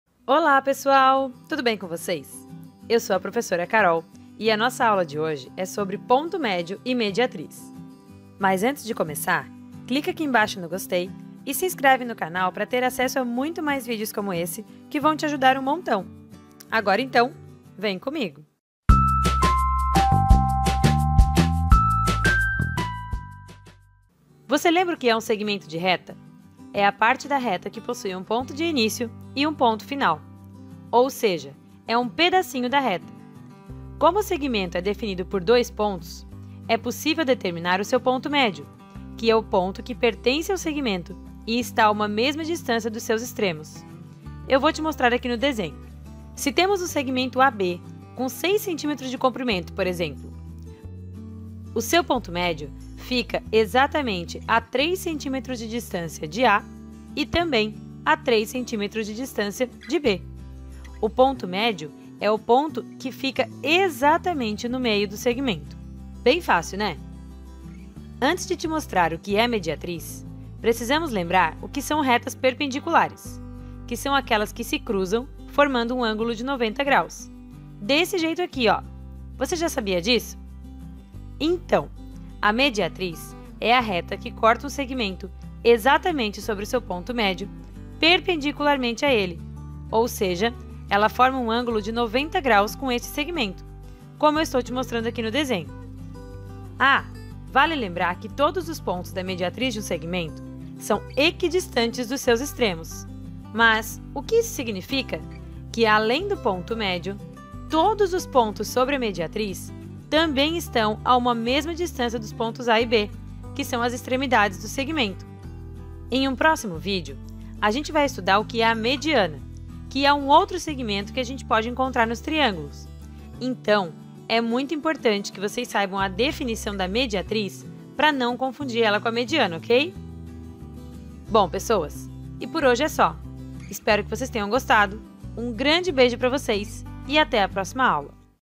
PONTO MÉDIO E MEDIATRIZ - Ensino Fundamental
Nesse vídeo eu te ensino o que são o ponto médio e a mediatriz de um segmento.